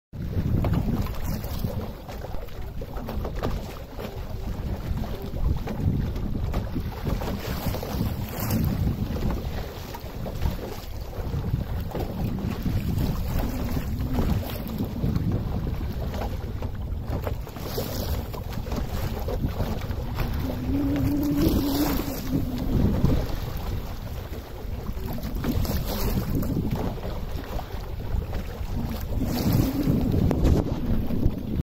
Sailing on the Pyhäselkä lake
Sailing on the Pyhäselkä lake in northern part of Saimaa lake district. In fact, Saimaa is one big lake consisting many parts.